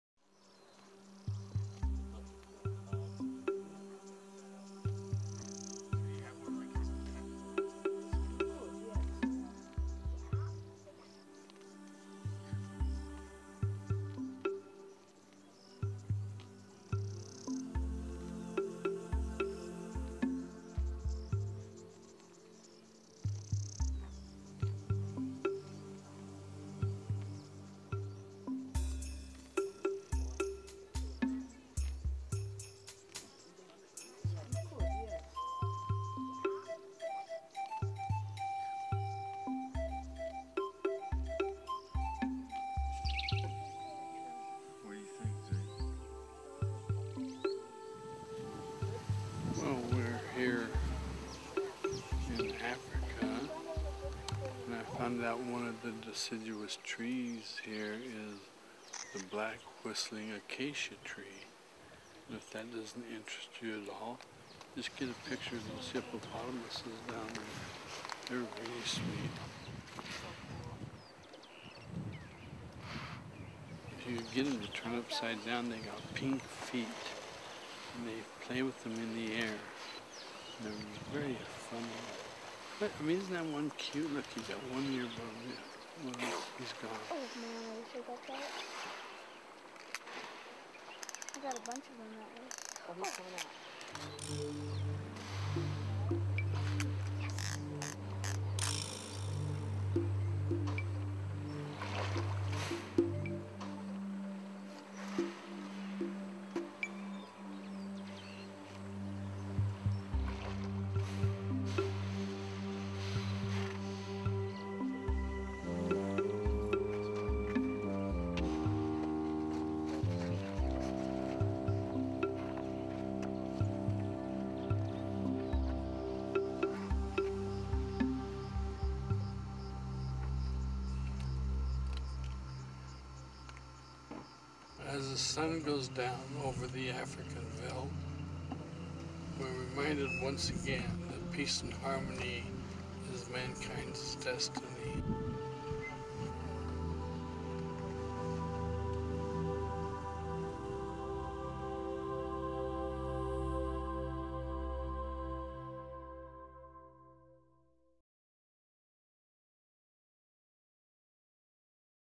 MSIA | John-Roger In Tanzania, Africa with the Hippopotamuses
MSIA Founder John-Roger shares in Africa how we are reminded that peace and harmony is mankind’s destiny.

MP150 A Moment of Peace - Ngorongoro Crater, Tanzania, April 2002
RUN TIME: 2:42

John-Roger, DSS

J-R: Well we’re here in Africa, and I found out that one of the deciduous trees here is the black whistling Acacia tree, and if that doesn’t interest you at all, just get a picture of those hippopotamuses down there, they’re really sweet.  If you get ‘em to turn upside down they got pink feet.  And they play with them in the air.  And they’re very funny. I mean, isn’t that one cute? Look, he’s got one ear above the other.  Oh, he’s gone.

Child’s voice:  You got a bunch of them that way.

(Music and background noises of rustling and water splashing.)

J-R:  As the sun goes down over the African hill, we are reminded once again that peace and harmony is mankind’s destiny.